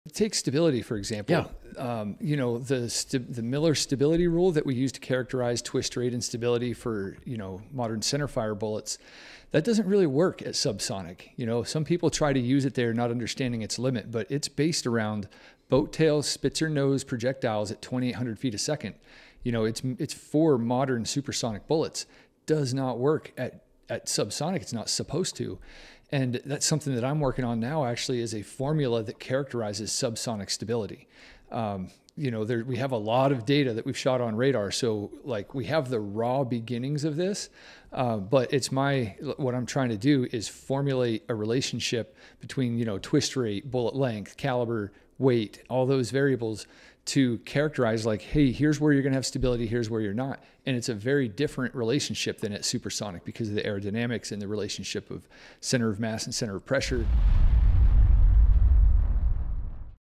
A New Formula for Airgun Projectiles
This short is from podcast #116 The Purpose of Airgun Research

Francis and Bryan discuss and explain the importance of testing airguns, along with 22lr and subsonic centerfire projectiles. Subsonic flight is characterized by challenges in projectile stability and rapidly changing drag conditions as you approach the speed of sound. They highlight the main reasons that airgun testing are beneficial to the overall understanding of subsonic projectile flight.